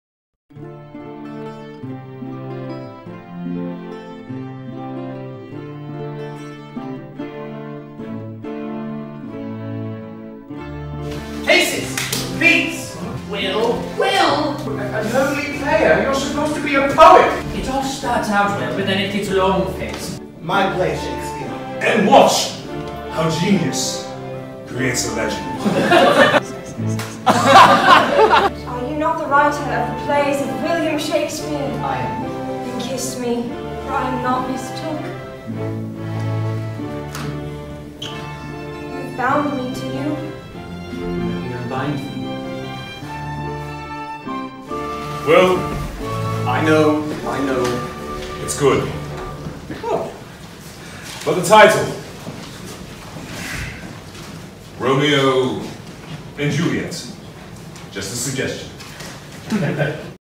SHAKESPEARE IN LOVE Designer Run Teaser Trailer - Palo Alto Players
PENINSULA PREMIERE!
Palo Alto Players' production of
SHAKESPEARE IN LOVE
January 18 - February 3, 2019
at the Lucie Stern Theater in Palo Alto